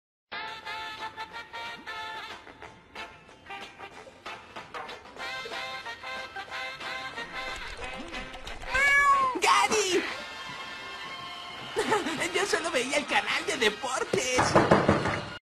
Yo solo veia el canal de deportes! (Loquendo City Parodias)
Me tarde un poco en pensar que video hacer para iniciar el año (2023), y pensé en hacer un remake de un video parodia del 2016 que yo hice en PowerPoint (Si, mis videos más viejos loquendo fueron hechos en PowerPoint :-p). Es corta esta parodia, pero espero que lo disfruten.

Mientras, me iré a terminar demás videos que tengo desde el 2022, más un video sorpresa :3